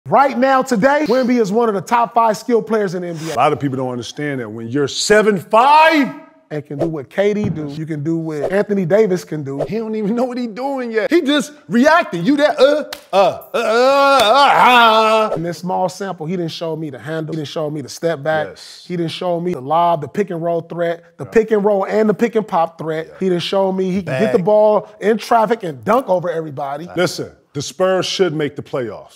KG & Paul Pierce BOLD Statement on Victor Wembanyama🏀
Join me, Kevin Garnett, as I team up with Paul Pierce for a BOLD discussion on Victor Wembanyama, one of NBA's top skilled players right now. In this short video clip, we delve deep into Victor's game style, his step back, his handle, and his pick and roll threats. We explore how at 7'5", he's changing the game, doing things KD and Anthony Davis can do, and making a splash in the NBA. We're talking skills, we're talking strategy, we're talking raw talent! Don't miss out on this insightful conversation as we put the spotlight on this emerging basketball giant. Remember, this is just a small sample of the full conversation. Witness for yourself the depth of our discussion and the magnitude of Victor's potential. Use to join the conversation and share your thoughts. Let's get this conversation started!